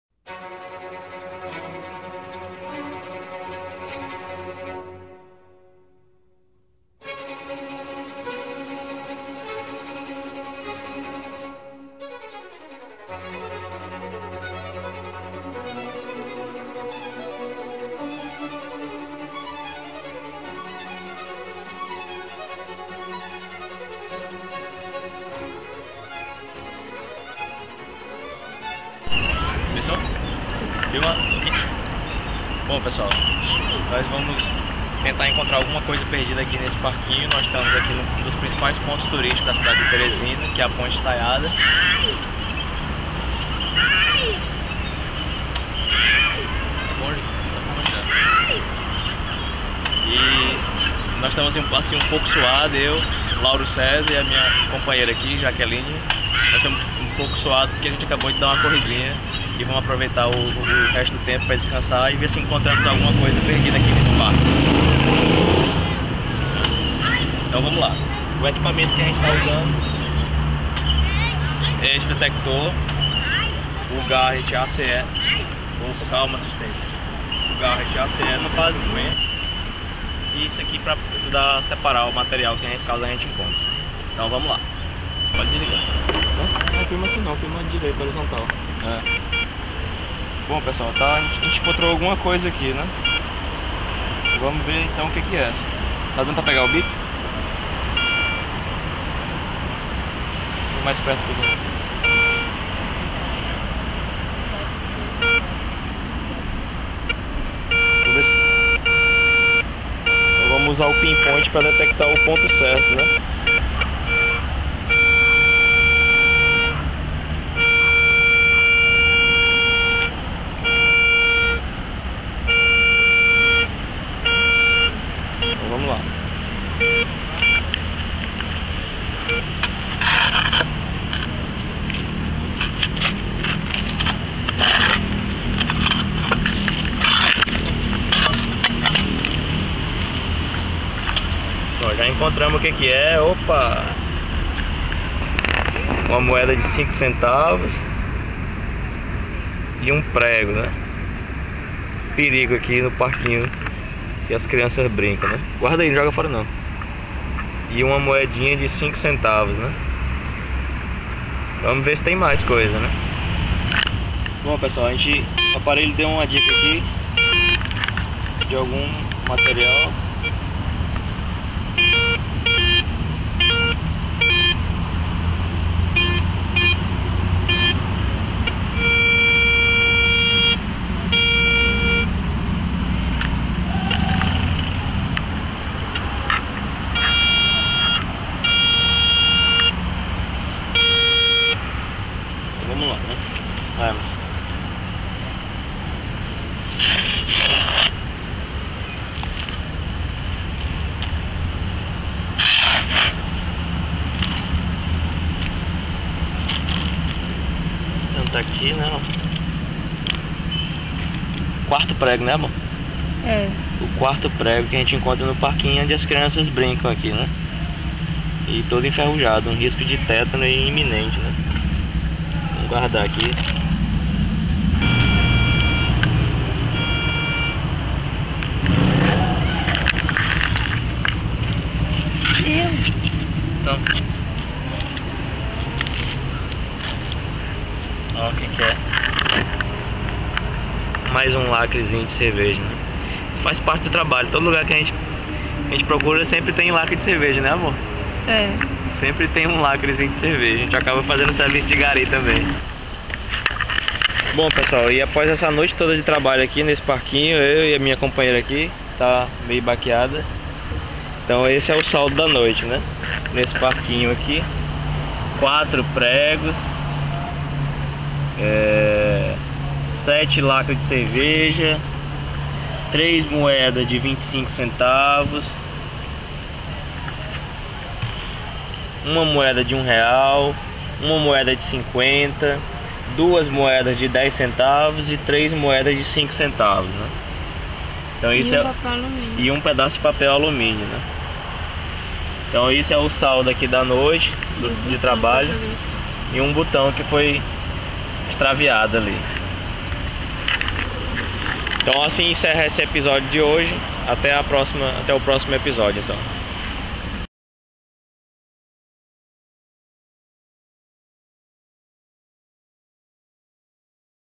Episodio 1 Caçada no parquinho da ponte estaiada
Testando o aparelho Garret ACE 250